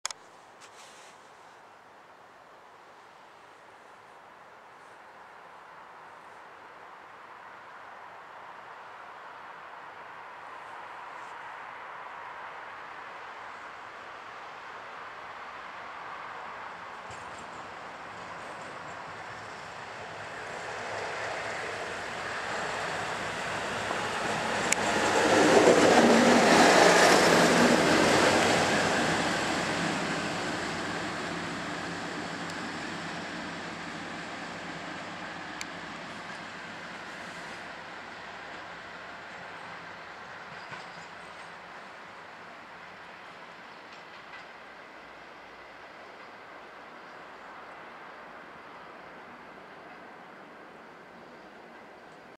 56 312 at Maidstone East
In a late night snow shower, 56 312 ARTEMIS passes through Maidstone East on 0Z56 Washwood Heath - Dollands Moor 16 minutes late at 22:07.